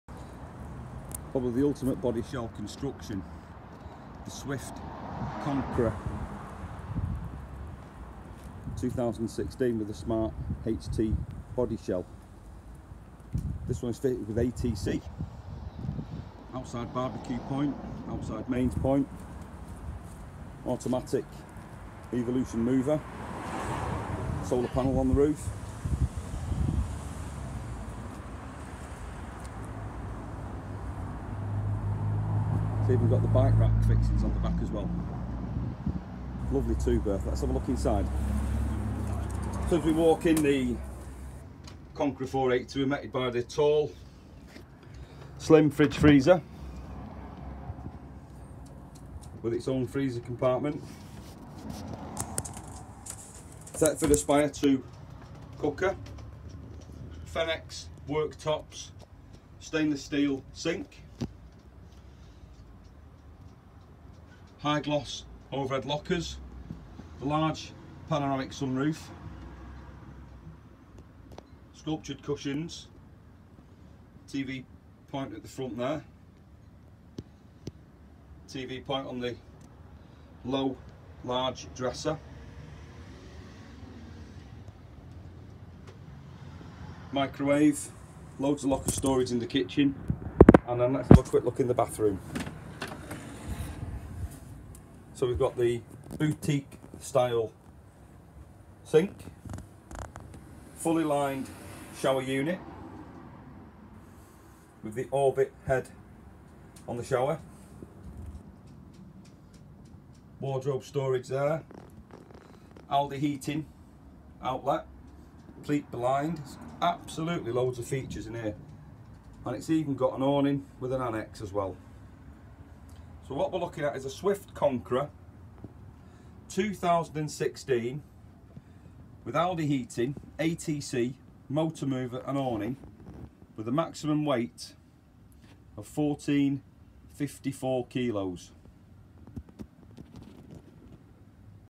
2016 swift conqueror 480 2 berth rear washroom for sale at North Western Caravans £16250.
New features for 2016 models include:-
MAJOR CHANGE: SMART-HT construction system including hail-resistant GRP roof

New: Gloss smoked oak doors

New: Foam bedding construction

New: Increased length front beds/seating

New: 'Loretta' upholstery scheme

New: Kitchen FENIX worktop (scratch/heat resistant with anti-bacterial properties)

New: 'Orbit' Eco Camel shower head with on/off function

New: 80 watt Solar panel

New: One key locking system

New: Thule cycle rack bars on rear panel for ease of cycle rack fitment

New: Slim-line Dometic tower fridge in some models

New: Swift Command System with Bluetooth electronics to control heating & hot water via smartphone/tablet

Interior of the 2016 Swift Conqueror range:-
 Interior rear

Additional features over Challenger models include:-
SMART HT construction - Totally timberless bodyshell with patent pending elements and Hi-Tech components

Alde heating system as standard

AL-KO ATC trailer stability control

Passive infra-red alarm system with tilt sensor and key fob operation plus LED awning light has red alarm active indicator

Heavy duty corner steadies on all models

Omnivent three speed, two way roof fan

Back lit washroom mirror and illuminated kitchen splash back

Full width curtains

Other features on 2016 models include:-
GRP inner and outer skins to all panels including patent pending 5 layer composite floor

Patent pending aluminium and SRIM corner jointing system

Patent pending cold bridge located within the aluminium side framing to prevent condensation

Unique highly aerodynamic exterior bodyshell with curved GRP front panel, large panoramic sunroof and LED marker lights

Wide access gas locker door with single lever three point locking

‘Aspire MKII’ oven with dual fuel hob with 800W electric hotplate and 3 gas burners with electronic ignition

External BBQ point, 230v socket and shower point plus aerial point in battery box for satellite dish connection

Increased internal space offers long front beds with bolster cushions that remain in place and double as headboard

Fixed bolsters either side of front chest with integrated rear heating vents

Exclusive Duvalay 'Duvalite' mattress in fixed bed models, with lightweight memory fibre technology - hypoallergenic, excellent pressure relief and dustmite resistant

Mini and Midi Heki clear skylights

Shower bi-fold door retained on track for improved access with upgraded transit catch

ALDE radiator central heating and water heating with daily programming and choice of 1kW, 2kW or 3kW setting

Ventilated microwave oven with digital controls

8-Series Dometic 110 litre, electronic ignition fridge with removable freezer compartment, black effect finish and digital controls (190 litre in twin axles incorporating 35 litre freezer compartment)

Thetford C260 toilet with electric flush and 18 litre wheeled waste tank

High flow rate in-board pump with chrome finish brass taps for reliable, high performance water supply

Radio/CD/MP3 player with iPod connection with premium radio speakers

Low energy LED lighting throughout plus down lighting and ceiling header lighting and dimmable spot lights

Thatcham approved TRACKER fitted as standard

Swift Conqueror 480

Number of Berths
Overall Length
Overall Width
Awning Size
MTPLM
Single/Twin Axle

2
654cm  21' 5"
226cm  7' 5"
901cm
1454kg
Single

2016 Swift Conqueror 480 plan

The 2016 Swift Conqueror 480 is a two berth layout with parallel front seating converting to two single beds or one double bed, centre kitchen and a spacious washroom with separate shower cubicle.